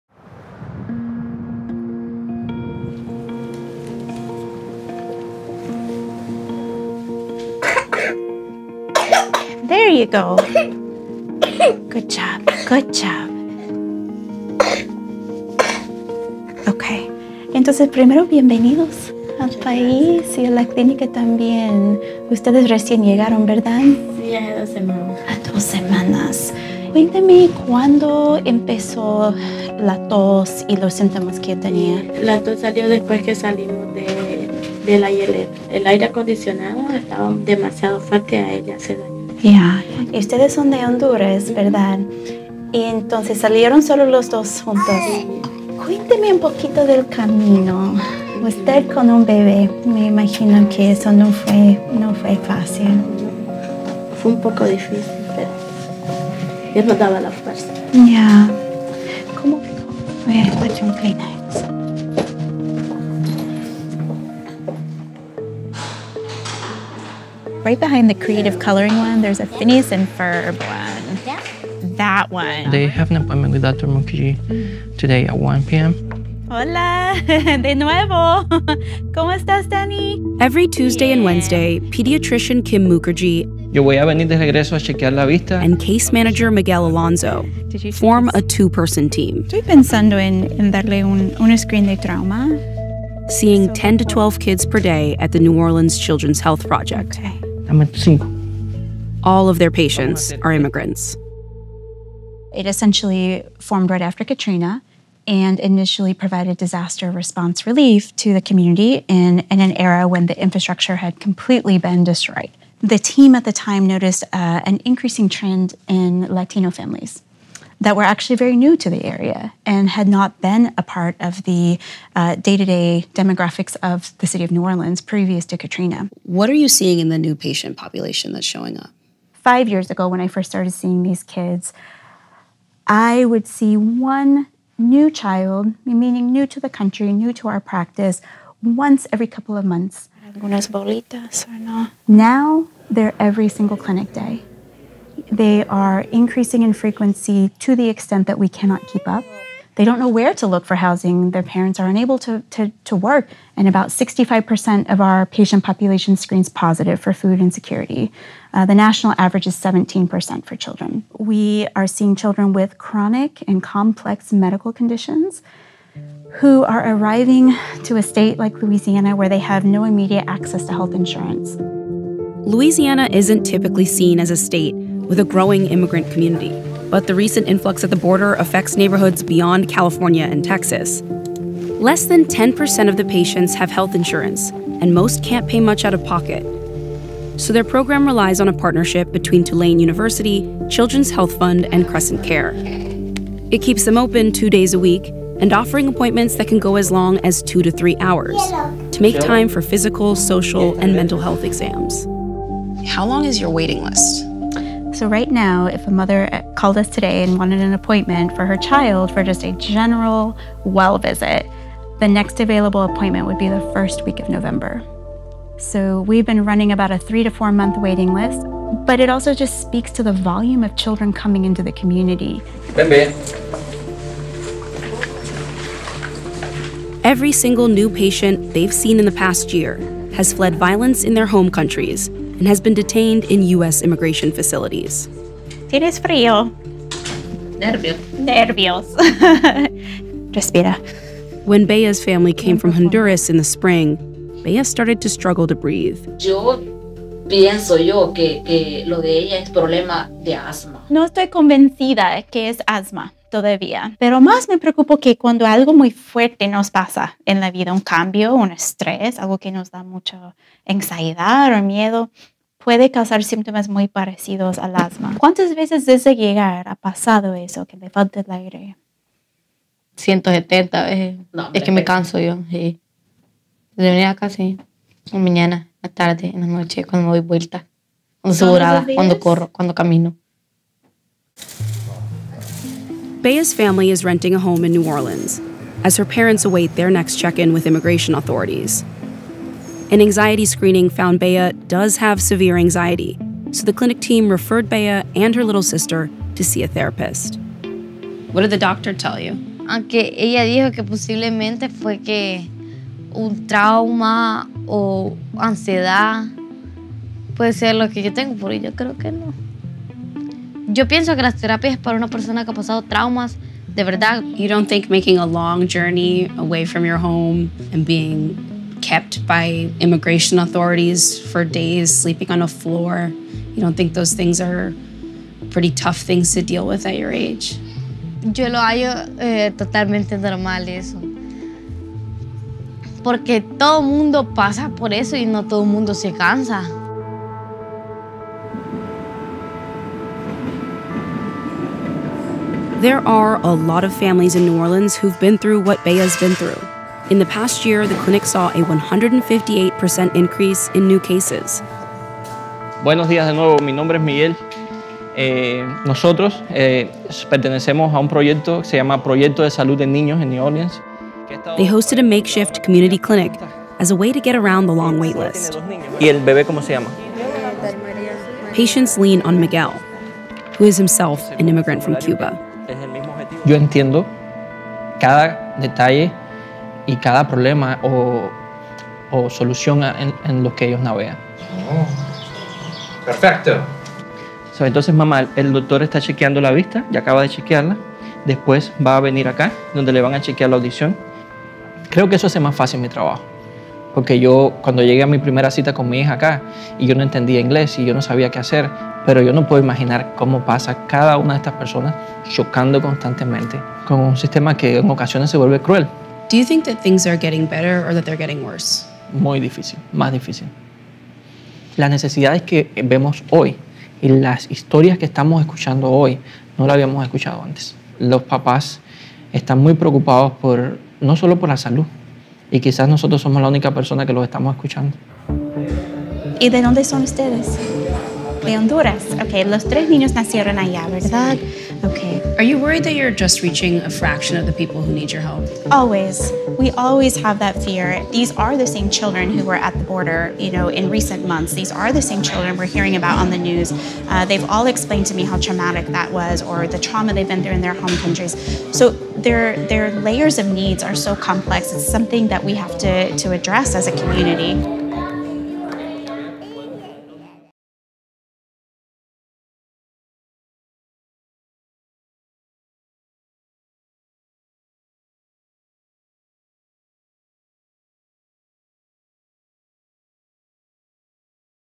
This New Orleans Clinic Was Created to Help After Katrina, Now Their New Patients Are Undocumented
Immigrants' kids are in serious need of healthcare. One clinic in New Orleans is trying to help.

Louisiana ranks 11 out of 50 for incoming unaccompanied minors, according to the Office of Refugee Resettlement. The New Orleans Children’s Health Project reported a 158% increase in new cases this year and a waitlist for new patients that's four months long, on average. VICE News traveled to New Orleans to meet the clinic team and their patients.